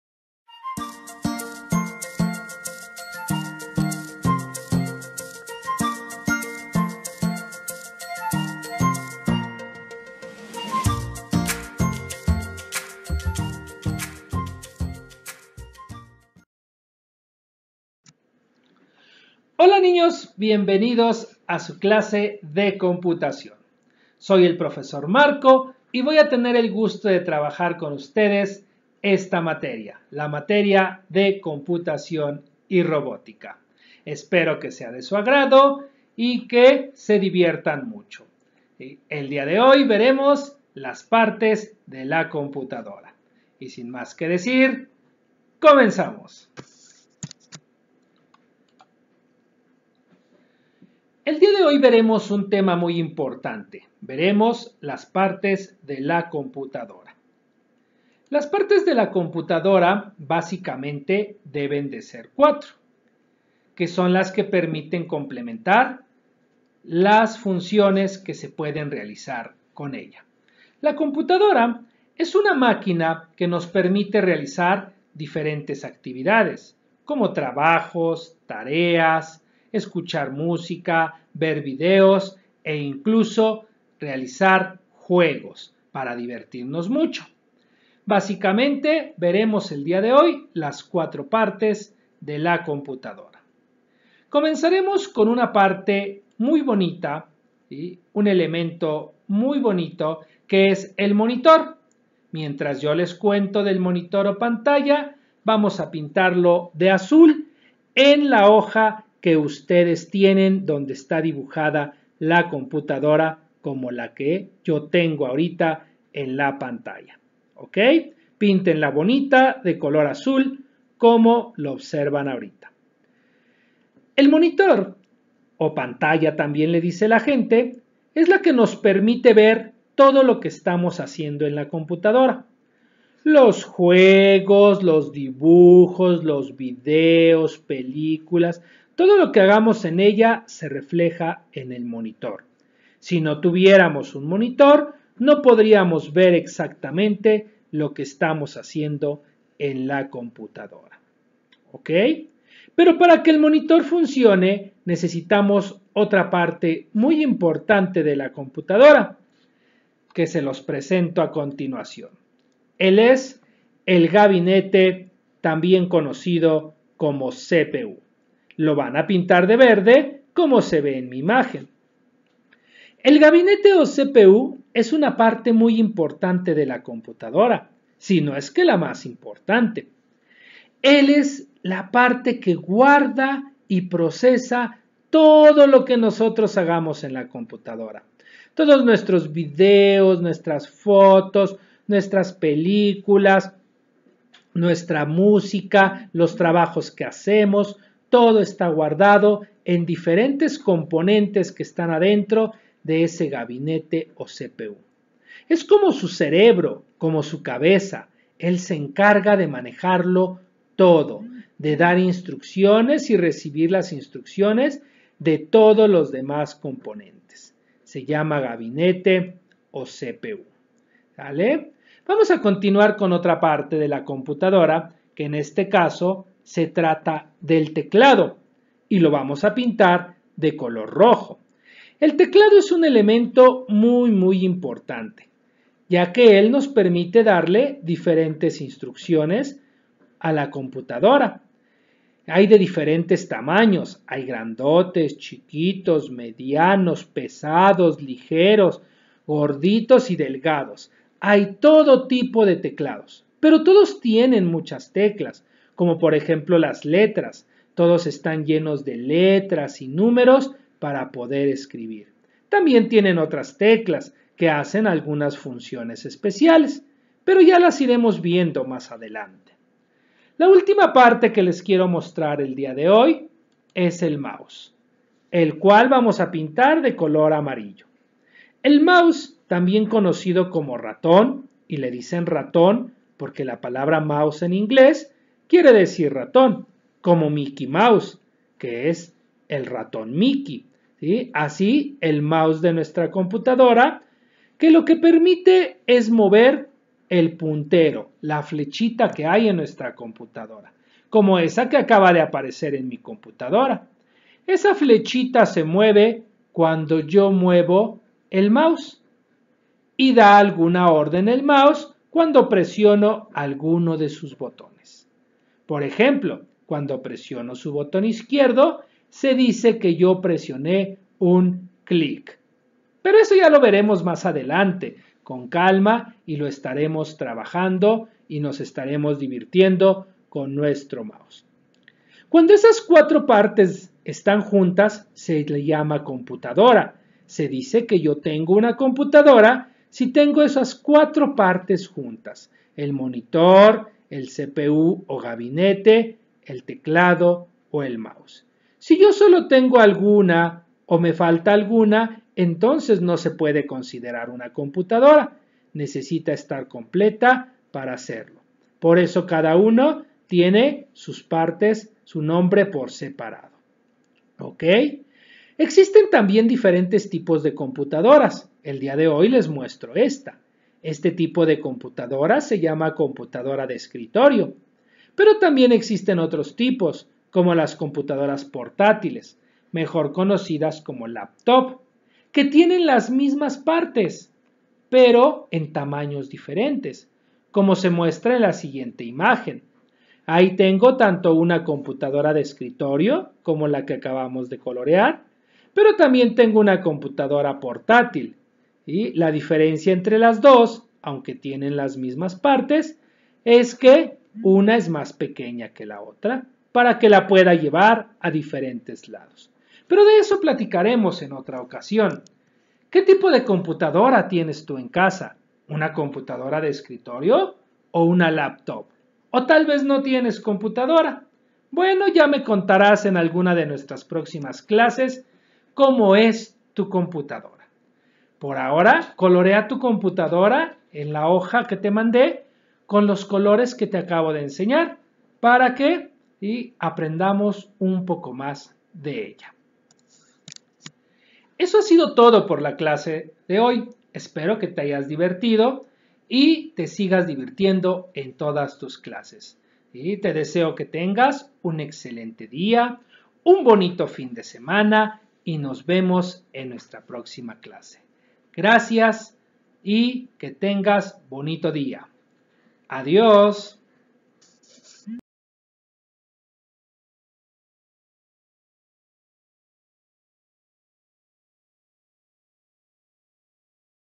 CLASE DE COMPUTACIÓN 3° DE KÍNDER - VIERNES 28 DE AGOSTO DEL 2020
CLASE DE COMPUTACIÓN 3° DE KÍNDER: Partes de la computadora.